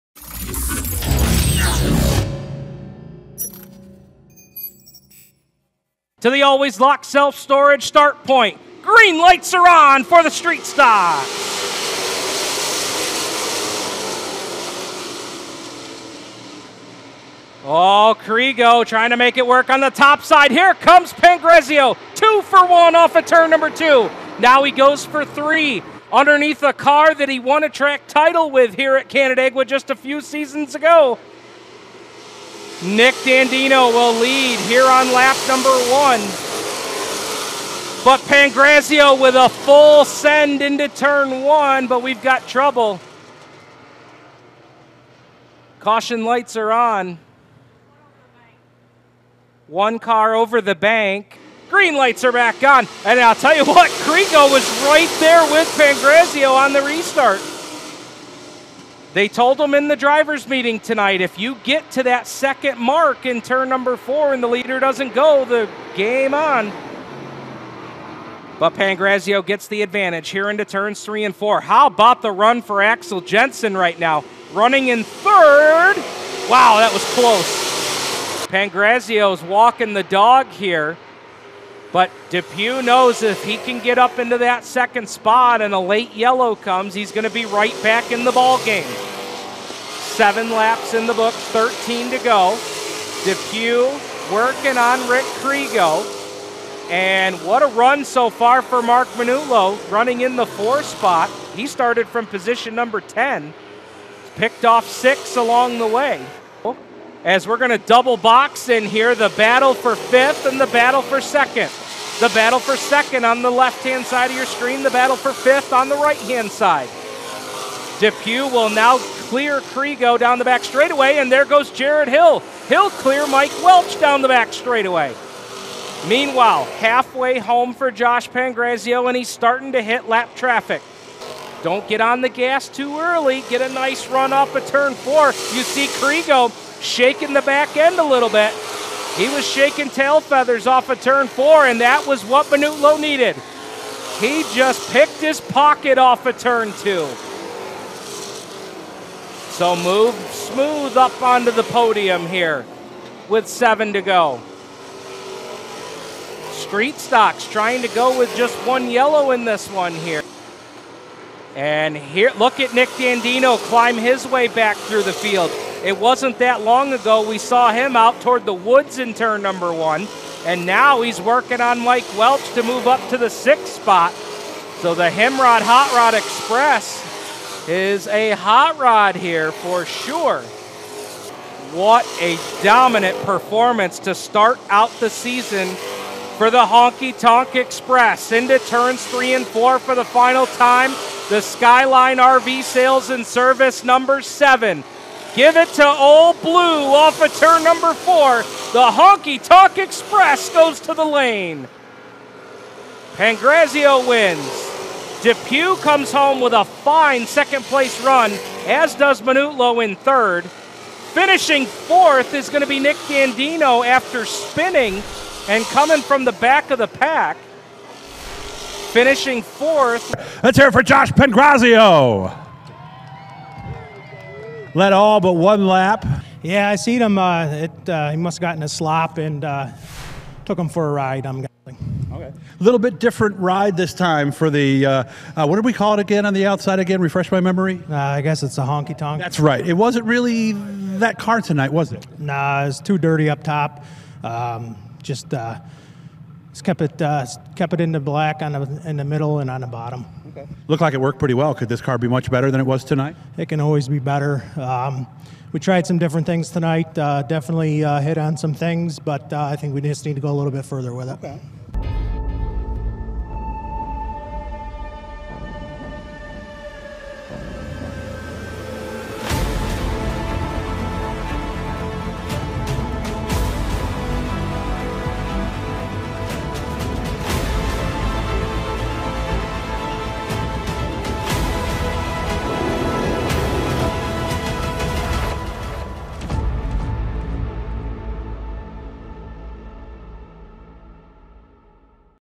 Eldredge & Son's Scrap Street Stocks Highlights April 30th
"The Honky Tonk Express" with Josh Pangrazio at the wheel left absolutely no doubt in the first Eldredge & Son's Scrap Street Stock feature of 2022. Pangrazio had the lead by Lap 2 and drove away in a race that only saw one caution flag. Even with the dominance of the 7, the racing behind Josh was superb!